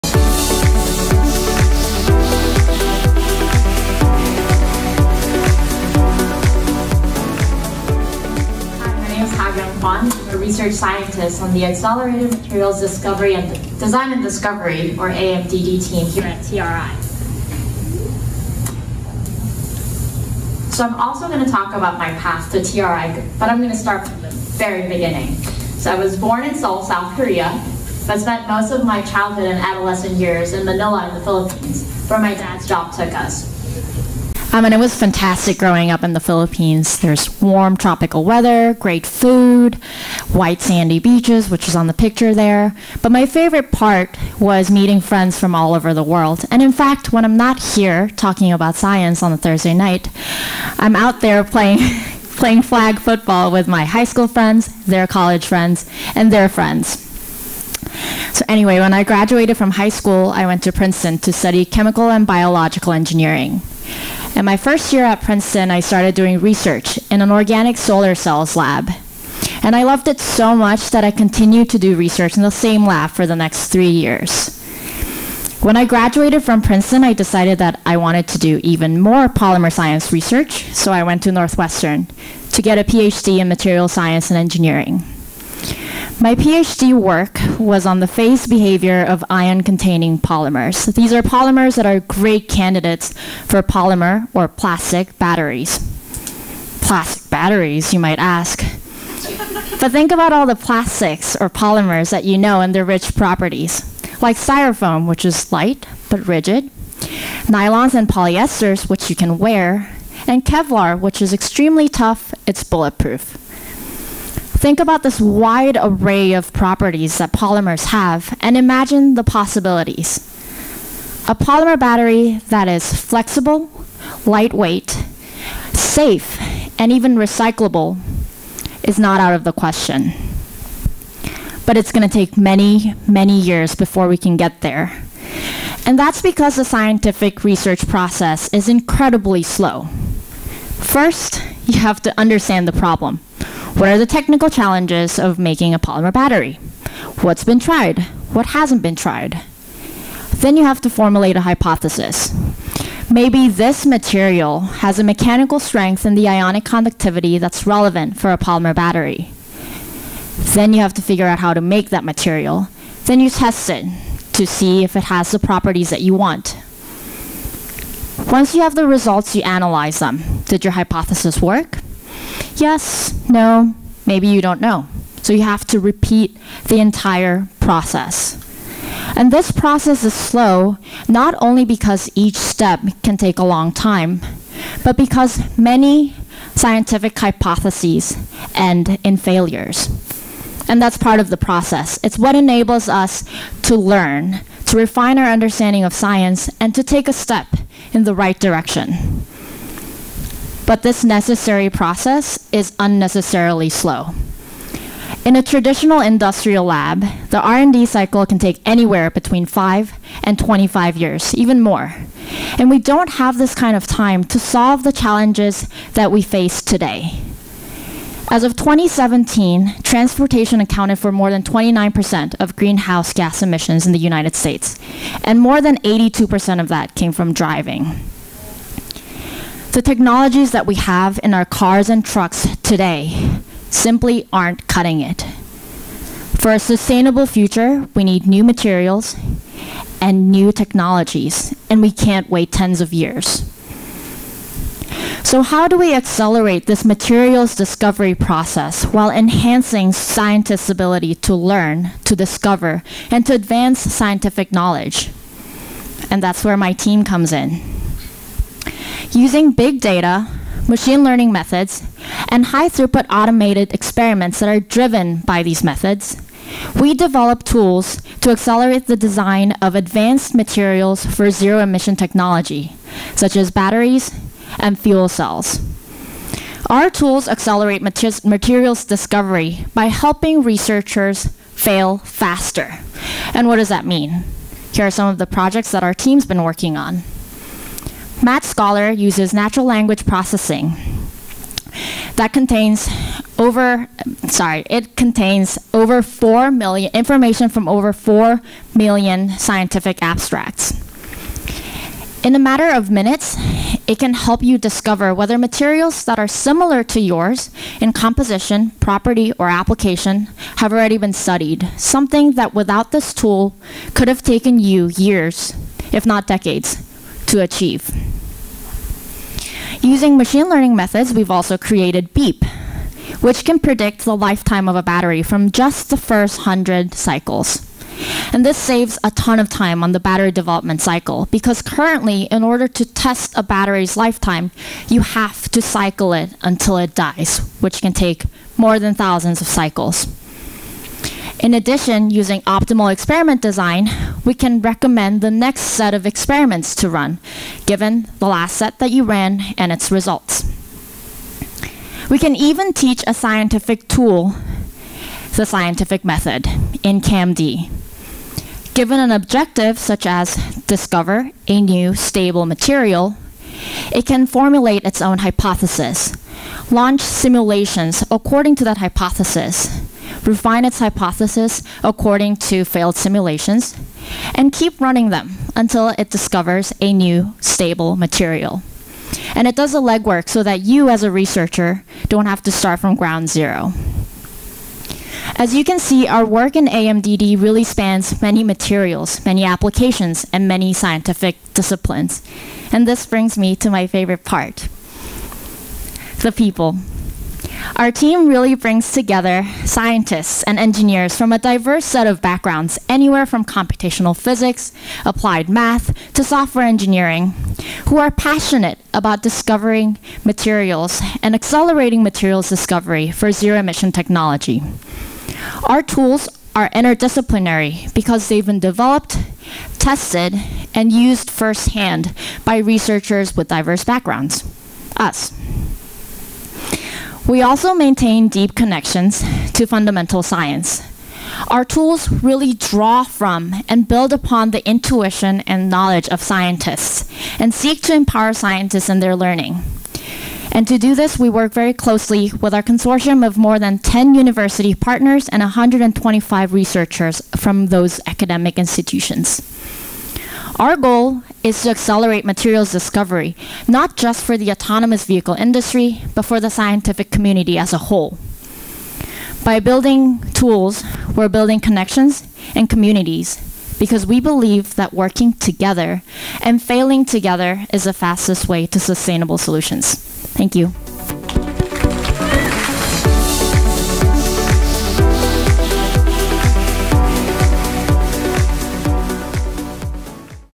Accelerating Materials Discovery by Helping You Fail Faster - Ha-Kyung Kwon, Research Scientist, TRI
As part of @toyotaresearchinstitute's Accelerated Materials Design and Discovery team, Ha-Kyung is building machine learning tools capable of rapidly advancing the discovery of new materials for batteries, fuel cells, and other critical components for use in future zero-emissions vehicles. She’ll share her insights about what makes TRI a unique place to work and her rare opportunity to couple disciplined and intensive research with cutting-edge machine learning techniques, which will enable humans to fail faster and, thereby, accelerate discovery.

0:00 Introduction
0:28 My path to TRI
2:17 Why I am at TRI
4:18 Accelerated materials design and discovery
4:38 Accelerating discovery by failing faster
6:36 Accelerating discovery by failing together

Talk by Ha-Kyung Kwon, Research Scientist, Toyota Research Institute